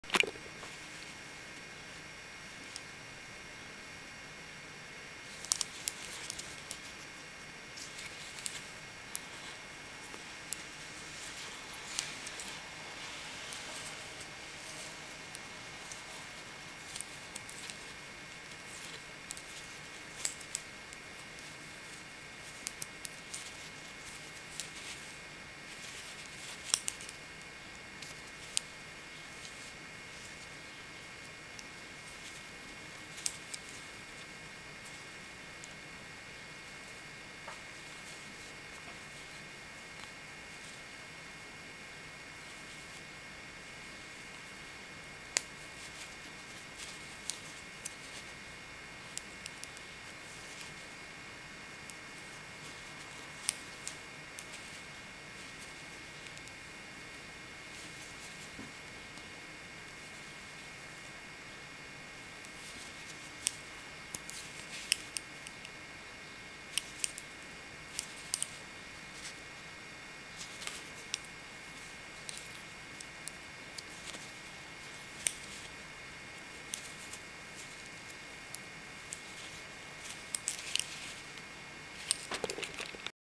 Ric Rac Stitch Part 1 of 2: The Knit Side Using Cabling Without Cable Needle Technique
How to work the knit side of a 2 Stitch Right Cross Cable without a cable needle.

This is a Stocking Stitch fabric with a simple 2 stitch cable cross without using a cable needle. The demo for the Purl Side is in second video.
Excuse the poor picture quality.